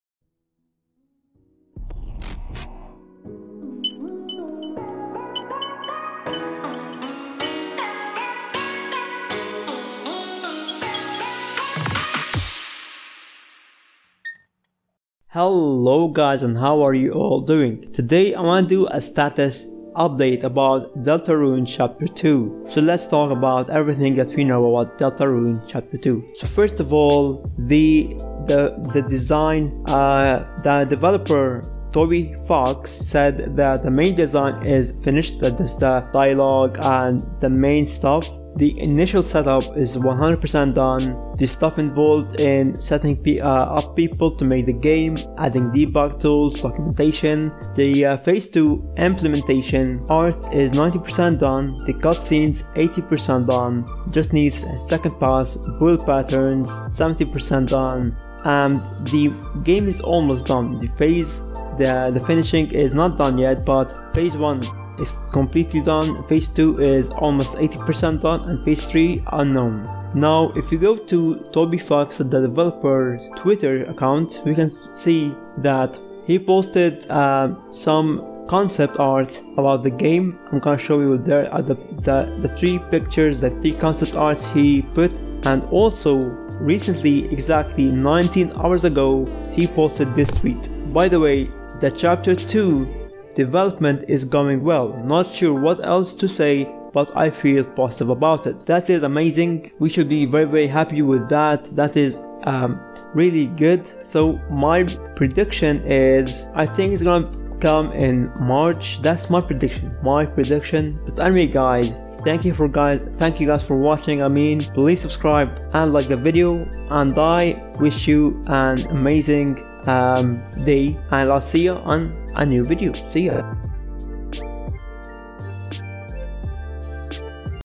DELTARUNE Chapter 2 Release Date, Development Plans And Developer Tweets (Toby Fox)
Undertale Creator Toby Fox celebrates Undertale and it's 5th Anniversary with the biggest Deltarune update yet! Toby Fox gives us the "Deltarune Status Update", which details the progress of Deltarune Chapter 2, as well as future Deltarune Chapters (DR CH 1, 2, 3, 4, 5, etc).  With more information than we have ever had to do with Deltarune, such as a further look at "The City", and some new screen shots showing gameplay of Kris, Susie and Ralsei in battle, with Toby Fox telling us the BTS of his development...